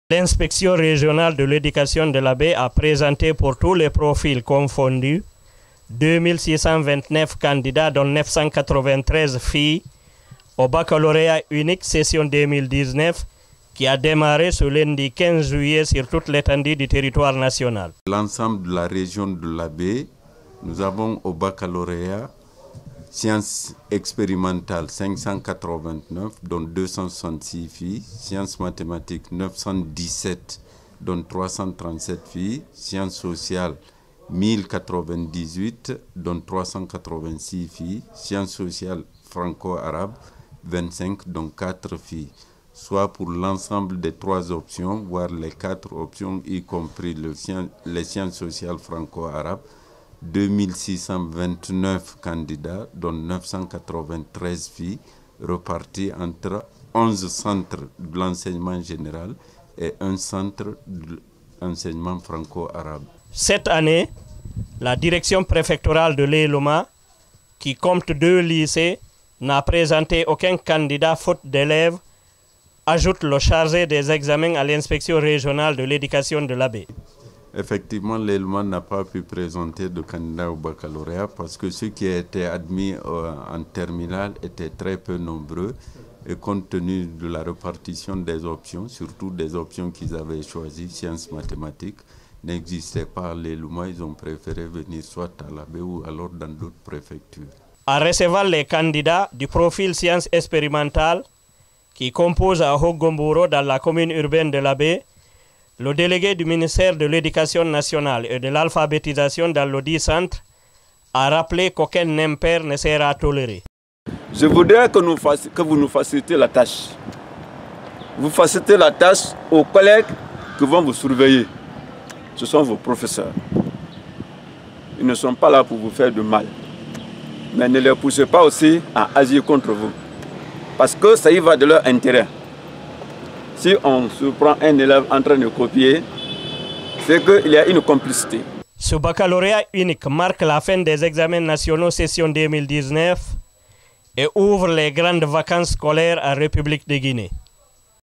Bac 2019 à Labé : 2 629 candidats dont 993 filles composent dans 12 centres
Au total, 2 629 candidats dont 993 filles composent dans 12 centres dont un pour l’enseignement franco-arabe dans la Région Administrative de Labé où la préfecture de Lélouma n’a présenté personne, faute d’élèves, rapporte un correspondant de Guineematin.com en Moyenne Guinée, à travers le reportage vidéo ci-dessous.